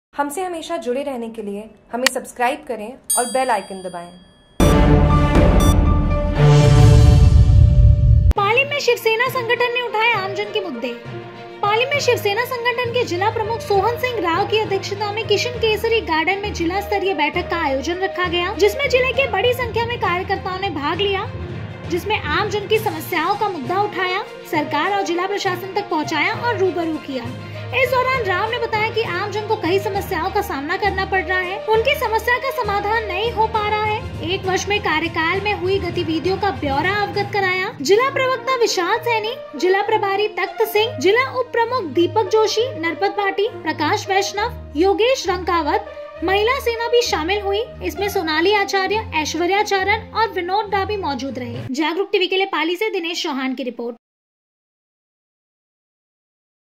Jagruk TV : पाली में शिवसेना संगठन ने उठाये आमजन के मुद्दे
पाली में शिवसेना संगठन के जिला प्रमुख सोहनसिंह राव की अध्यक्षता में  किशान केसरी गार्डन में जिला स्तरीय बैठक का आयोजन रखा गया। जिसमें  जिले के बड़ी संख्या में कार्यकर्ताओं ने भाग लिया। जिसमे आम जन की समस्या का मुद्दा उठाया। सरकार और जिला प्रशासन तक पहुचाया ओर रूबरू किया। इस दौरान राव ने बताया की आमजन को कई समस्याओ का सामना करना पड़ रहा है। उनकी समस्या का समाधान नही हो पा रहा हैं, एक वर्ष में कार्यकाल में हुए गतिविधियों का  ब्यौरा अवगत कराया । जिला प्रवक्ता विशाल सैनी, जिला प्रभारी तखतसिंह, जिला उपप्रमुख दीपक जोशी,  नरपत भाटी, प्रकाश वैष्णव, योगेश रांकावत, महिला सेना भी शामिल हुई इसमे सोनाली आचार्य, ऐश्वर्या चारण ओर विनोद डाबी  मौजूद रहे।

पाली से दिनेश चौहान की रिपोर्ट

Jagruk TV